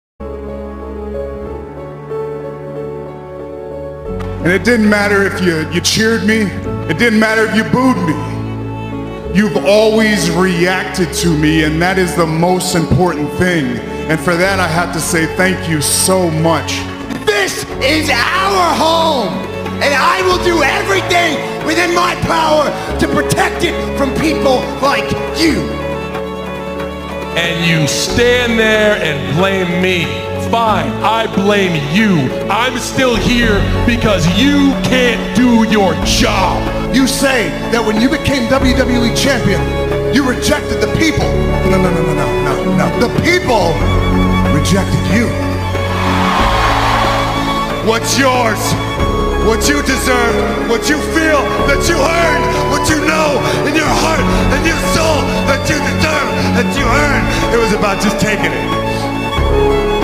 Times When Wrestlers Weren't Acting 😳 (Part 4)
in this edit I've shown times when wrestlers weren't acting but simply speaking the truth (Part 4). Namely : John Cena, Roman Reigns, Seth Rollins, The Rock, CM Punk & Dean Ambrose.